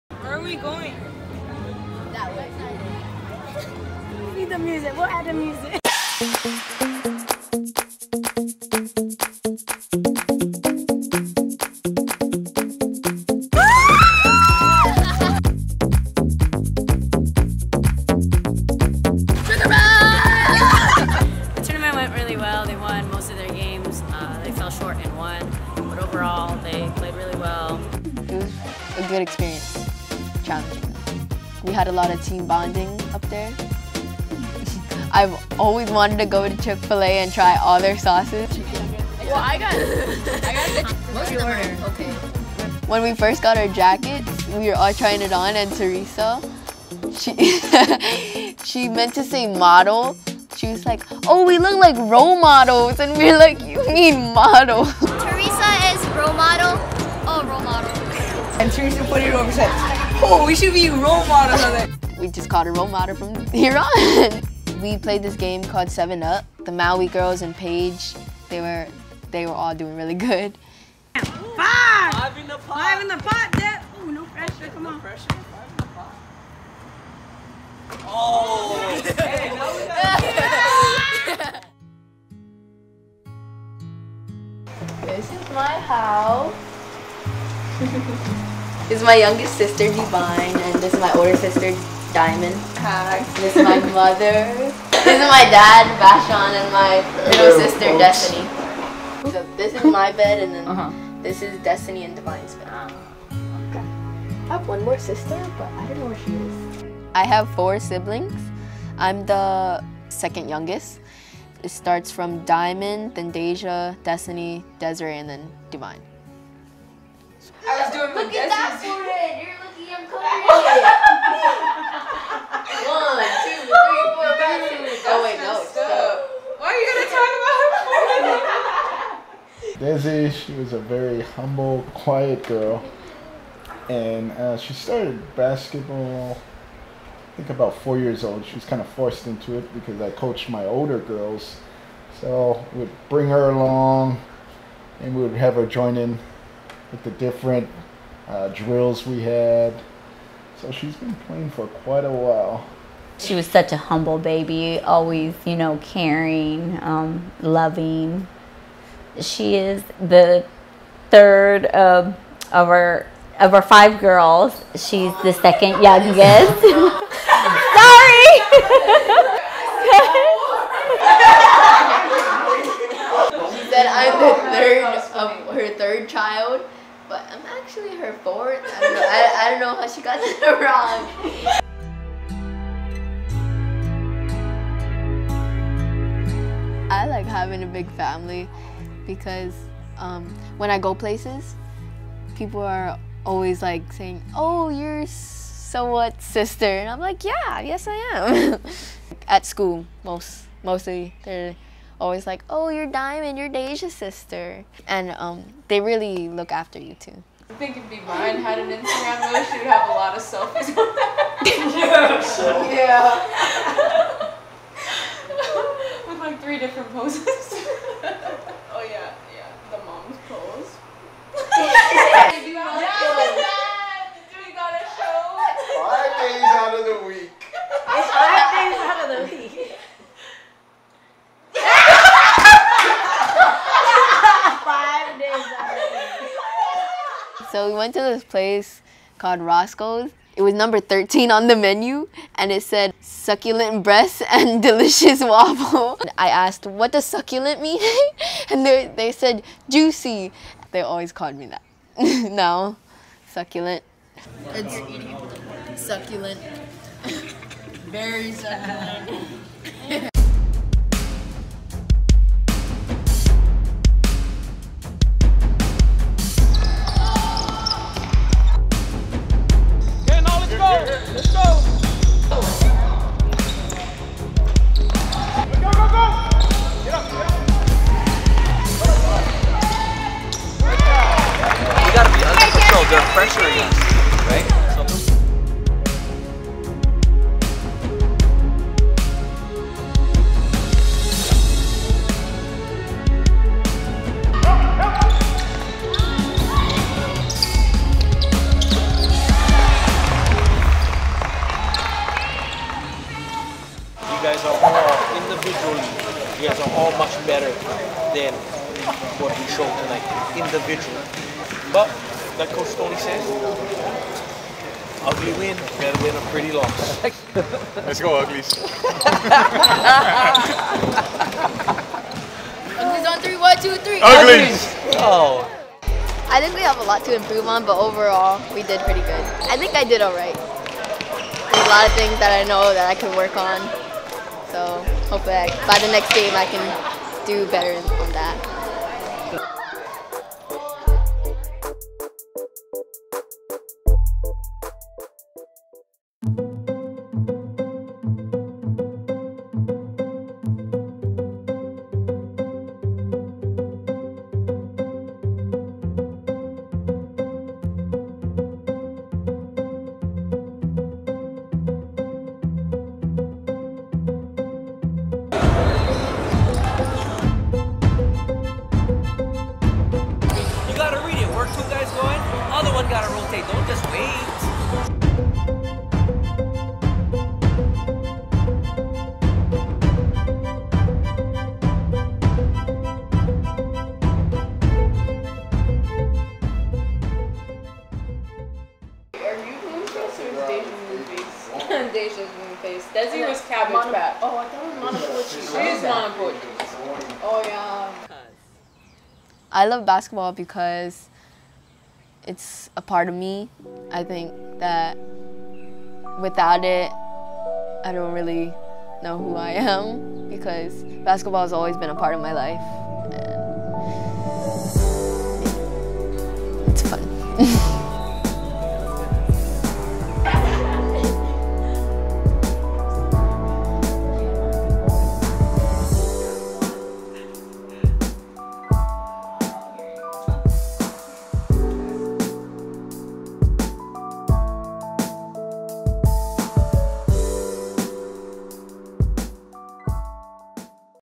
Lightning Girls Travel Season: "Dez" Episode 4
The girls play well at the Cali Tournament. Dez talks about basketball and family life.

Video produced by Jugga: @dajugga